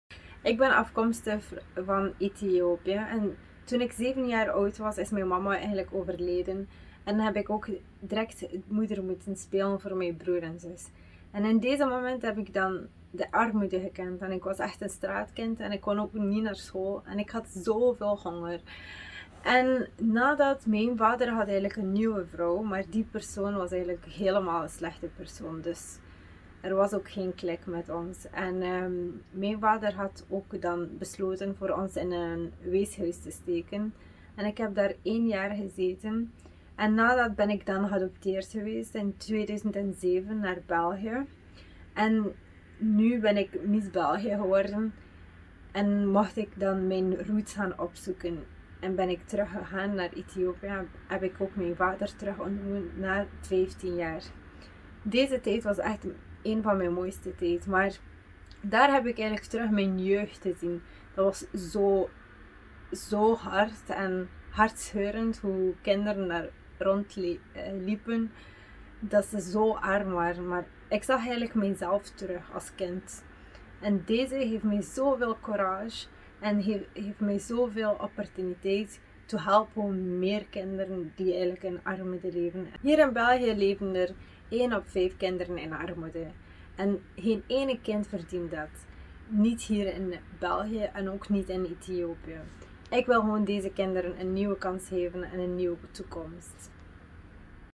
Fondation Pelicano - Ambassadrice, Kedist Deltour
Notre ambassadrice, Kedist Deltour, a connu la pauvreté quand elle était enfant. Elle a vécu dans la rue, elle n’a pas pu aller à l’école, elle n’avait presque jamais de quoi manger... 😔

Dans cette vidéo, elle vous raconte son histoire. Elle vous explique comment elle est sortie du cercle vicieux de la pauvreté et pourquoi nous devons nous entraider pour éradiquer la pauvreté infantile en Belgique. 😘